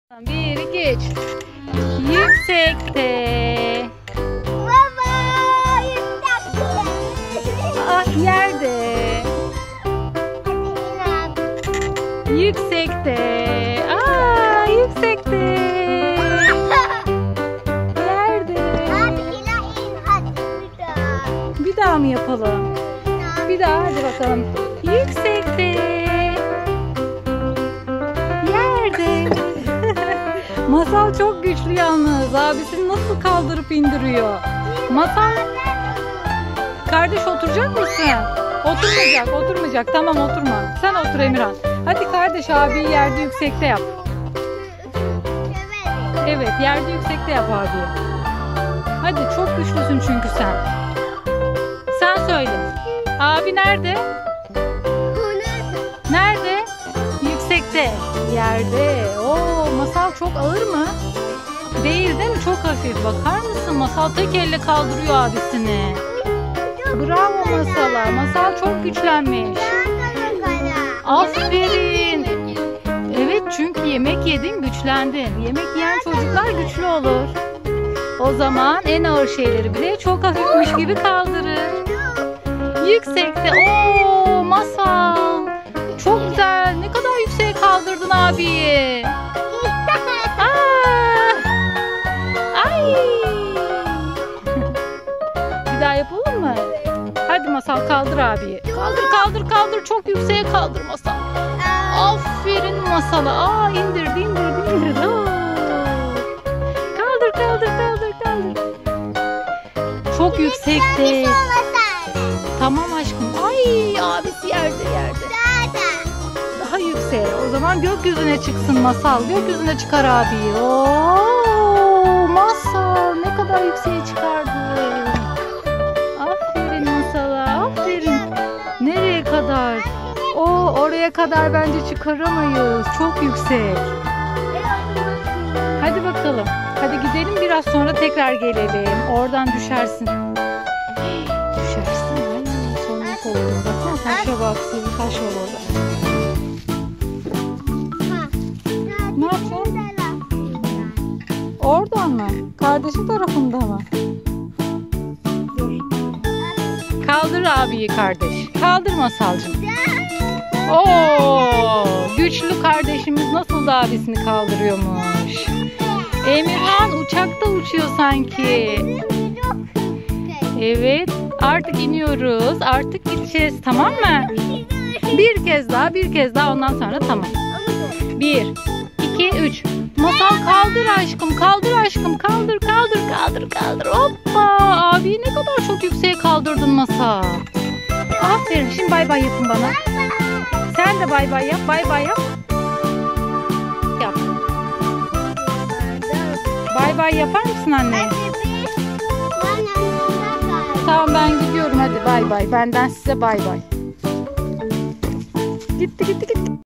Masal Tahterevalli İle Emirhanı Çok Yükseğe Kaldırıyor! Masal Çok Güçlü💪🏻💪🏻
Masal Ve Emirhan Ormandaki Parka Gitti!  Orada Tahterevalli, Kaydırak Ve Salıncaklar Var. Rengarenk Parktayız. Masal Emirhan Abisini Çok Yükseğe Kaldırdı! Masal Çok Güçlenmiş! O Çok Güçlü!